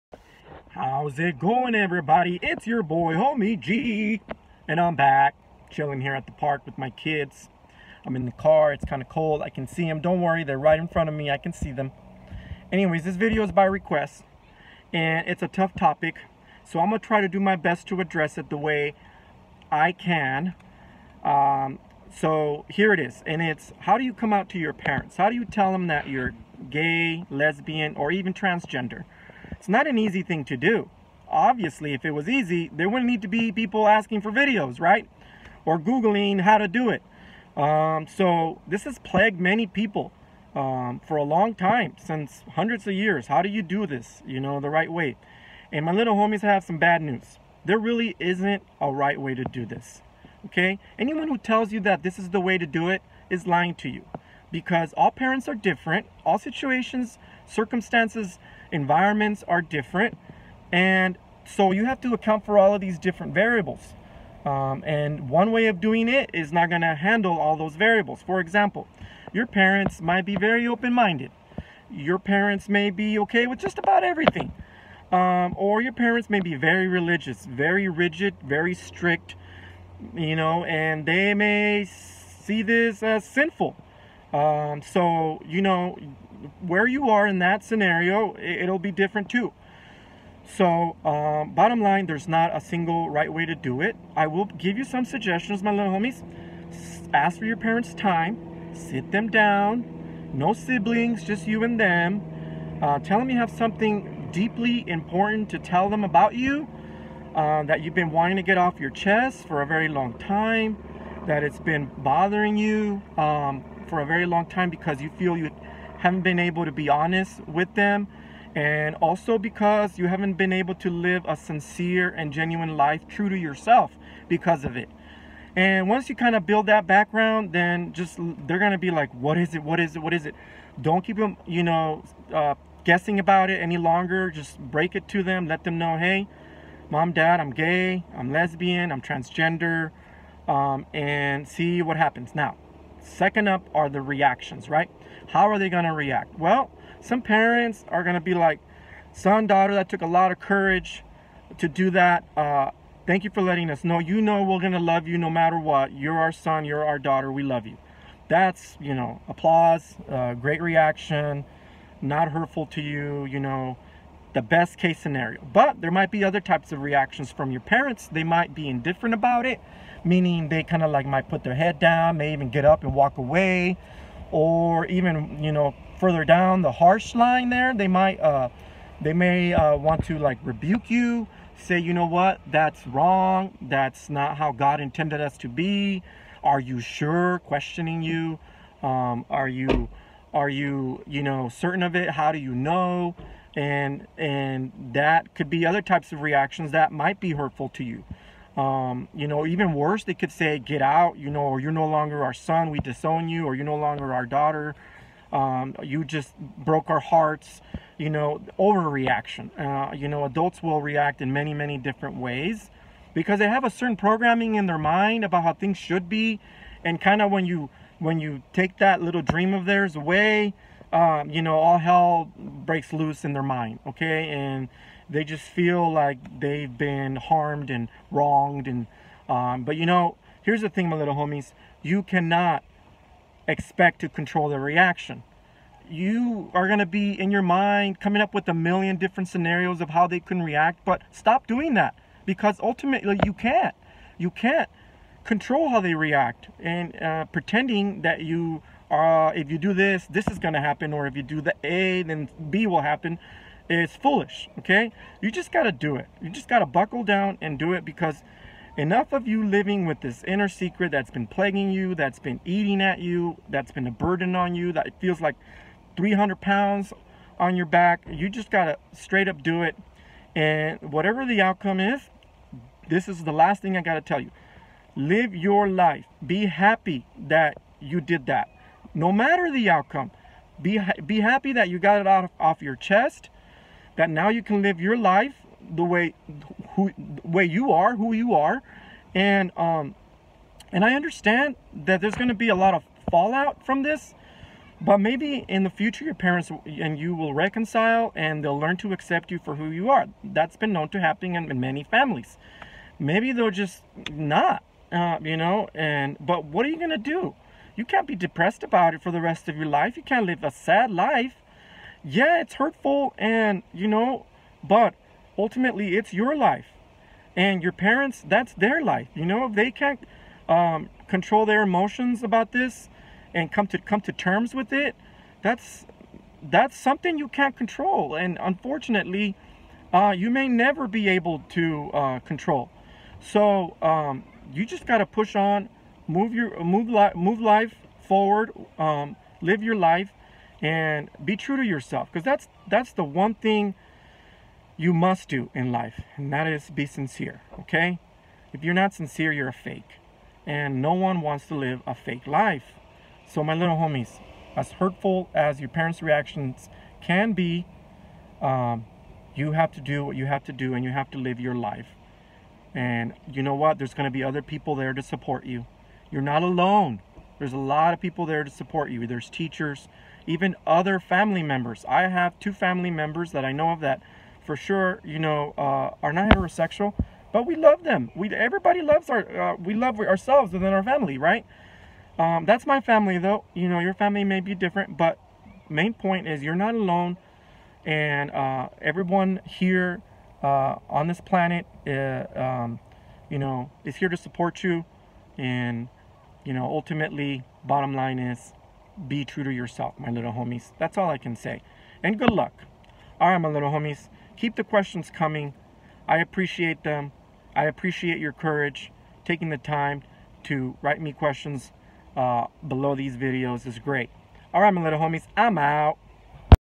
How to “Come Out” to Your Parents
In this vid, Homie G helps his little homies with some advice on how to let your parents know you’re LGBT. Also, how to deal with the reaction.